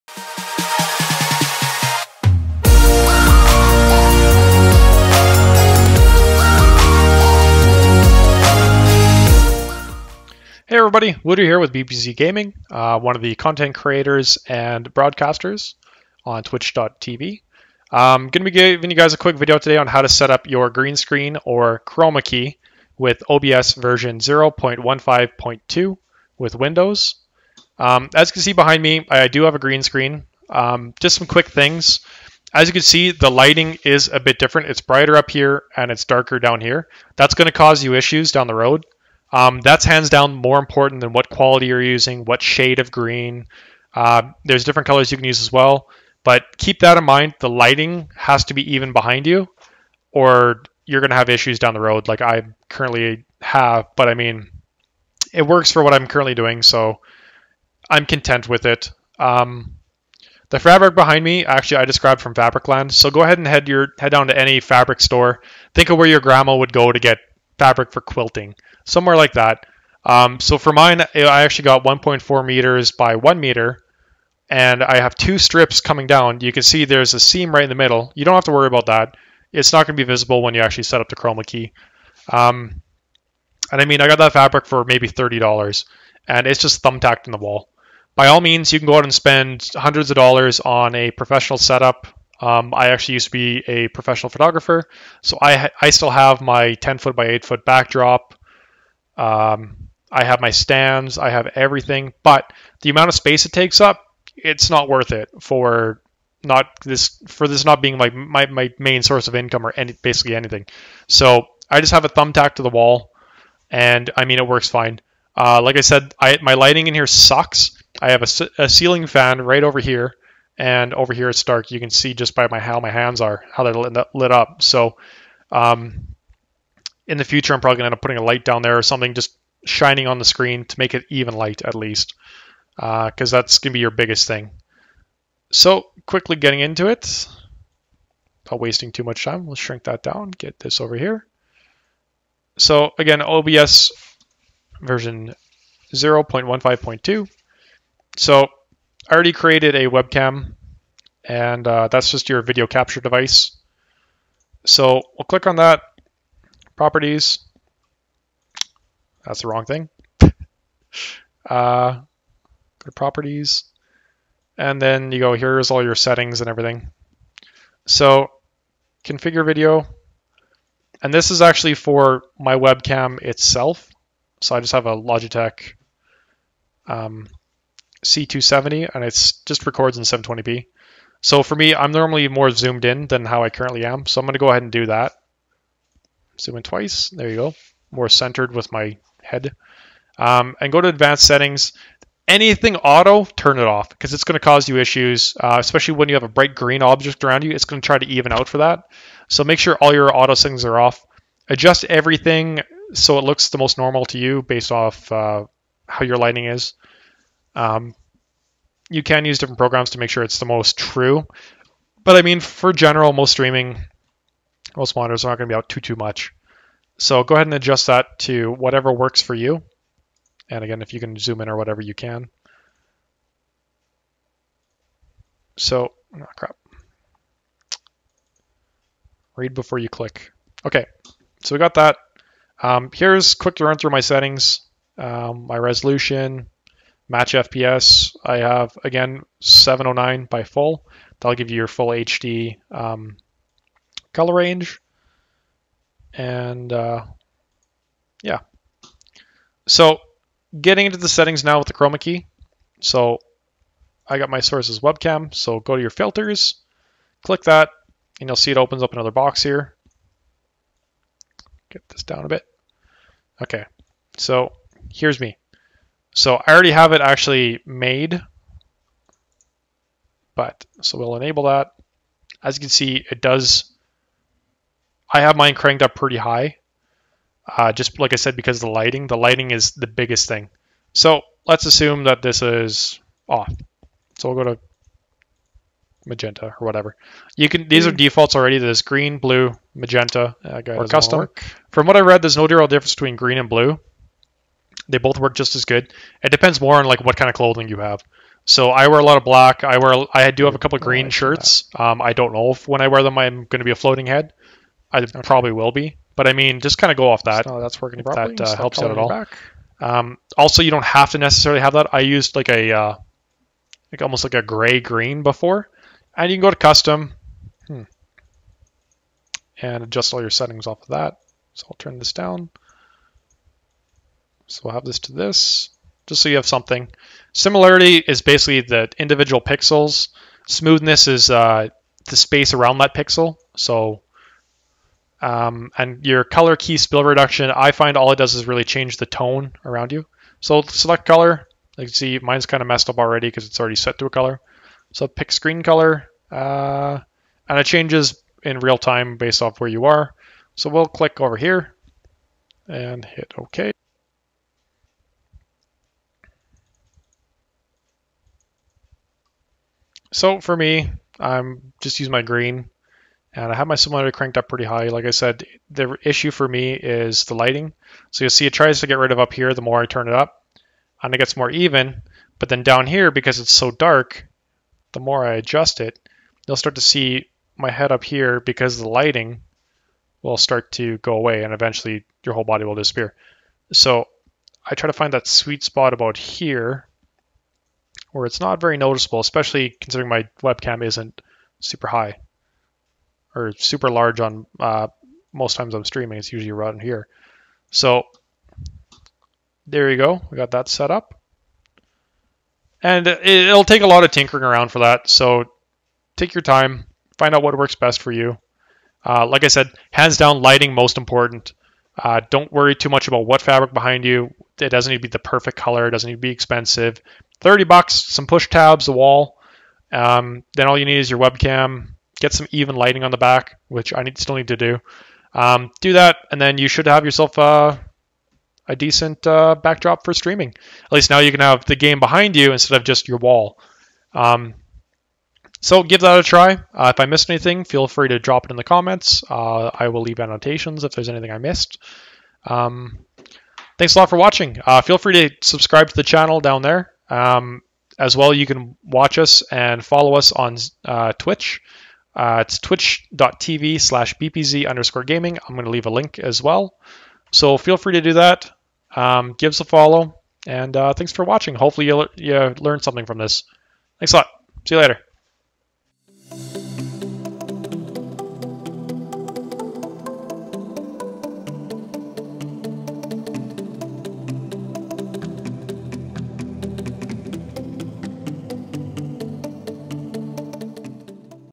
How to Setup a Green Screen in OBS Studio with Tips
Guide on how to set up a Green Screen, or Chroma Key, in OBS Studio v0.15.2. Total cost only $30 for a green screen! Make your games and streams look even better by not broadcasting your wall!

Links to our groups!